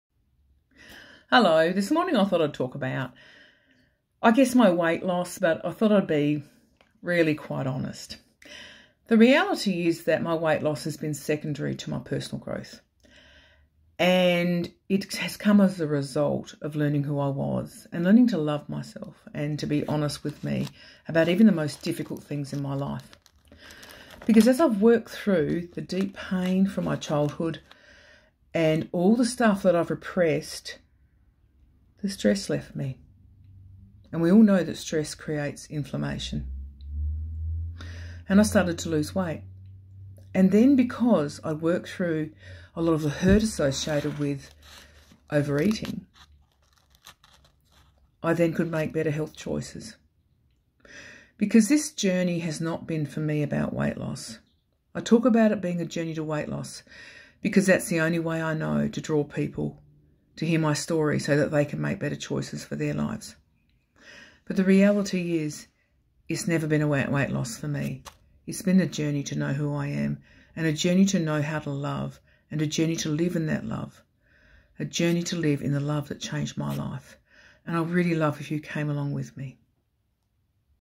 Jenny Marshall. Personal growth and learning to love myself have resulted in great weight loss.
I describe journey to health and how it resulted in weight loss of over 100kg. It began as a journey to know myself. As I understood who I was, I could love and respect me. My stress levels reduced and I could make healthier choices for my life. Learning what it meant to love myself and what love was and choosing to be motivated by it changed my life.